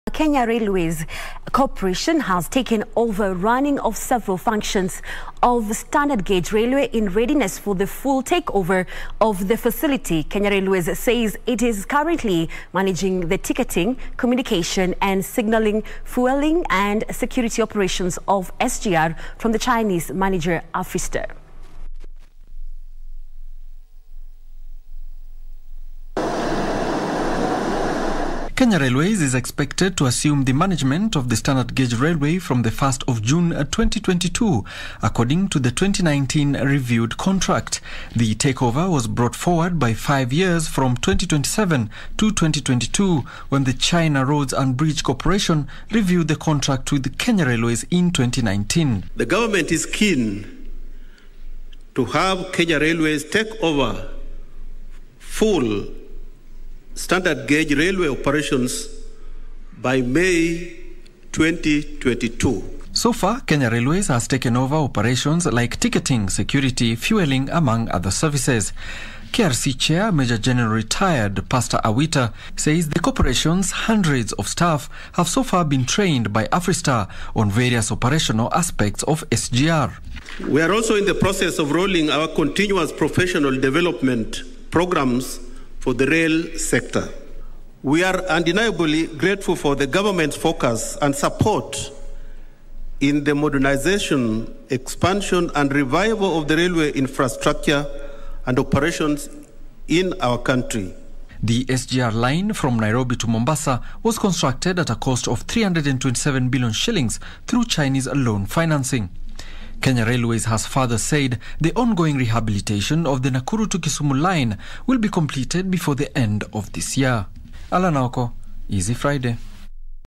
Kenya to take over SGR operations from June 2022
The Kenya Railways Corporation has taken over the running of several functions of the standard gauge railway in readiness for the full takeover of the facility. Kenya railways say its currently managing the ticketing, communication and signaling, fueling, and security operations of the SGR from the Chinese manager Afristar.